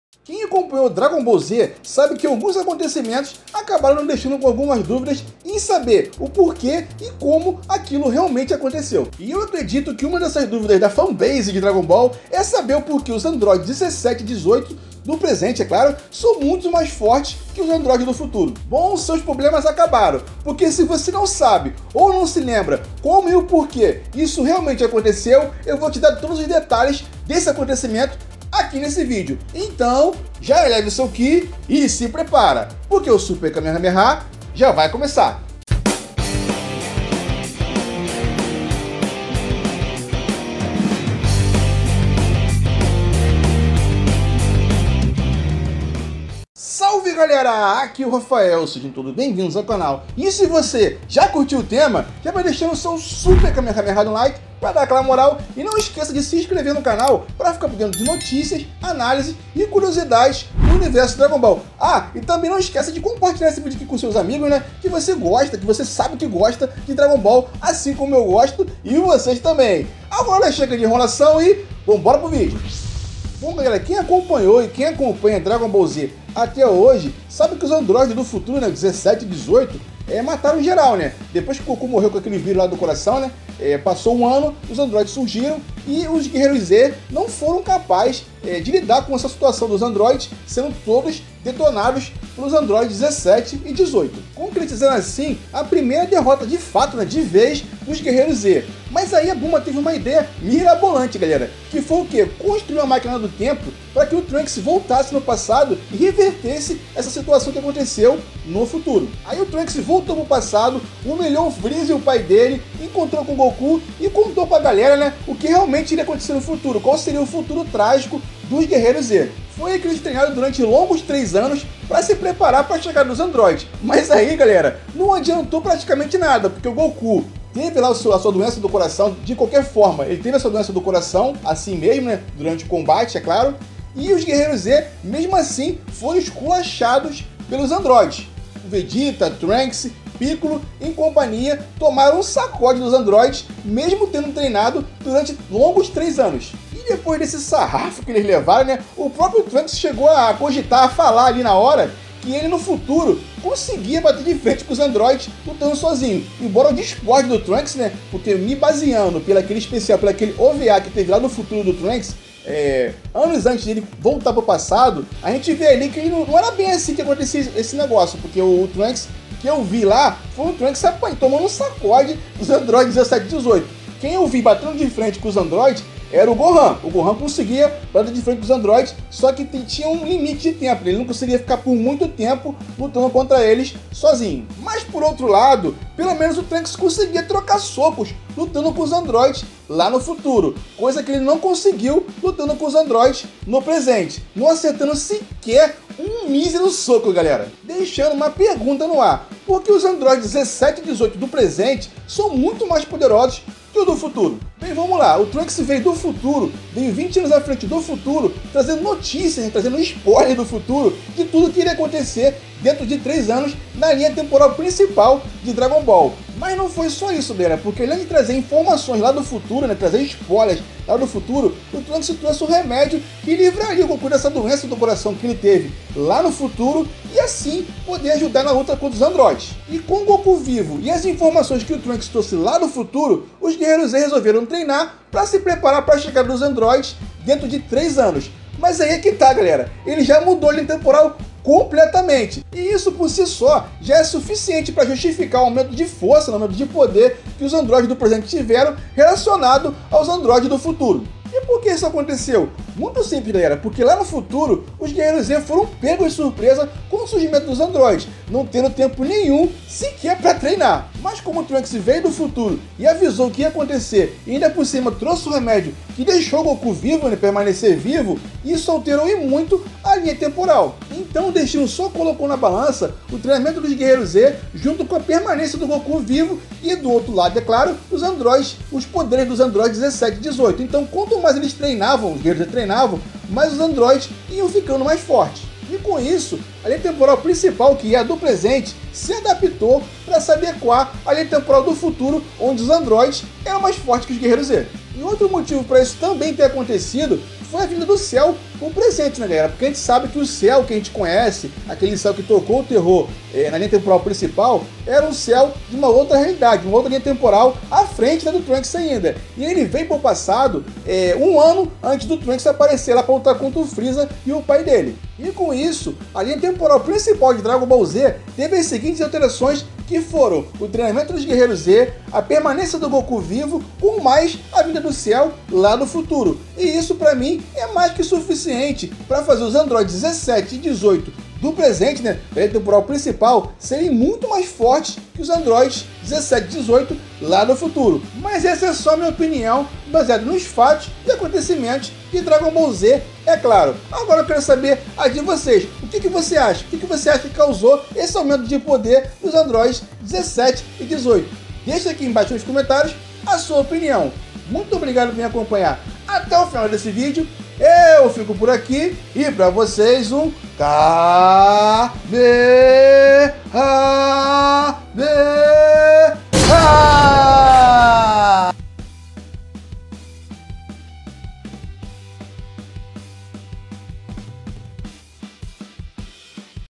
DBZ | ENTENDA PQ OS ANDRÓIDES 17 E 18 DA LINHA PRINCIPAL SÃO MAIS PODEROSOS!
Neste vídeo eu falo sobre o pq os Andróides 17 e 18 da linha principal de dbz são mais poderosos, usando fatos e acontecimentos do anime e mangá, e fazer com que você entenda pq os Andróides 17 e 18 da linha principal são mais poderosos!

[Um personagem épico] entenda porque Piccolo merece ser o protagonista de Dragon Ball Super: Super Hero